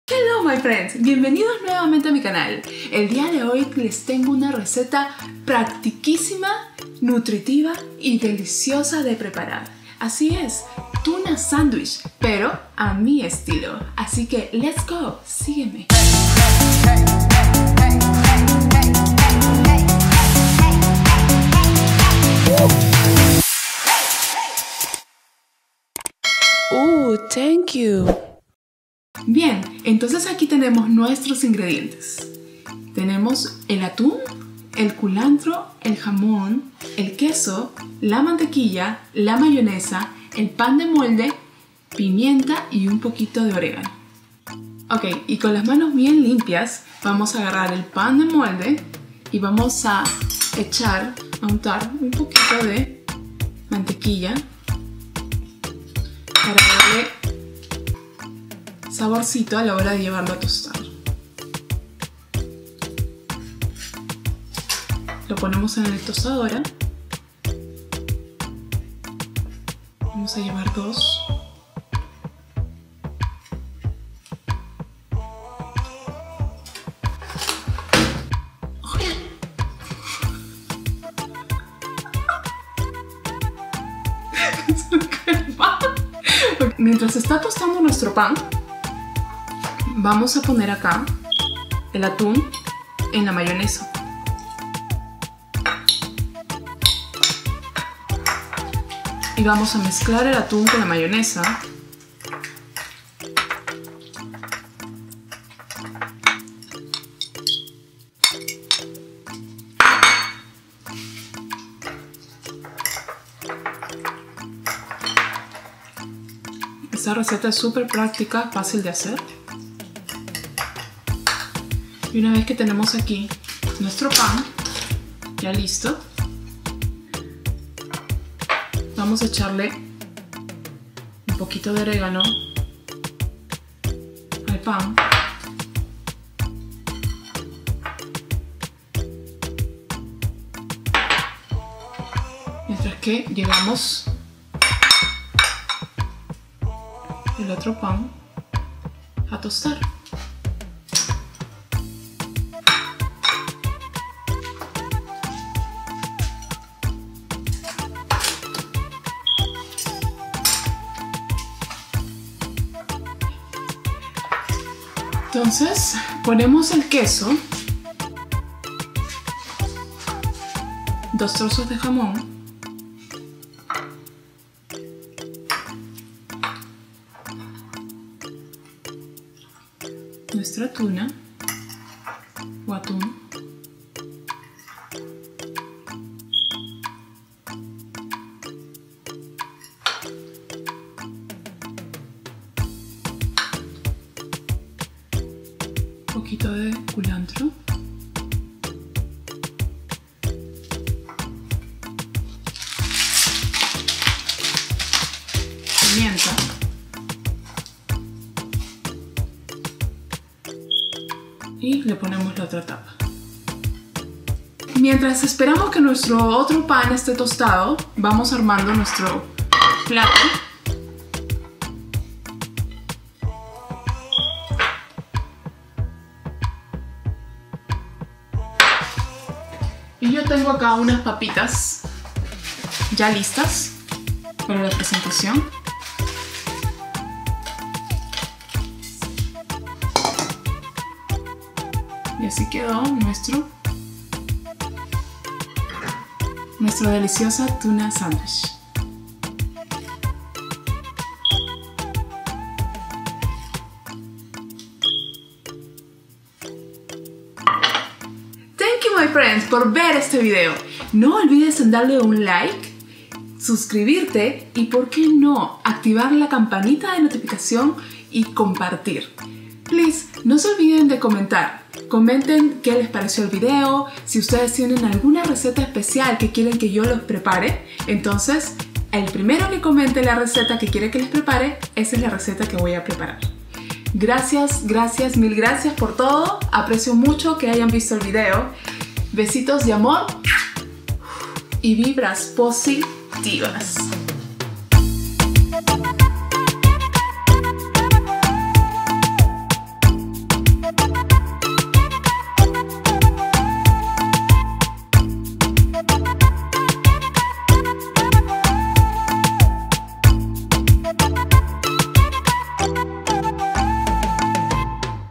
DELICIOSO : Tuna Sandwich : Sandwich De Atún!
En este video quiero compartir un sandwich preparado con mucho amor ya que es a mi estilo , una vez que lo pruebes te dejará encantado de lo delicioso que es , se volverá tu favorito sandwich ya que es nutritivo , delicioso y súper práctico de hacer.
Ven y disfrutarlo conmigo! Provecho .

Ingredientes :
Atún
Mayonesa
Jamón
Queso
Pan
Culantro
Pimienta
Orégano
Mantequilla

Producers:          Iska Plotkin & Jason Plotkin
Director:              Jason Plotkin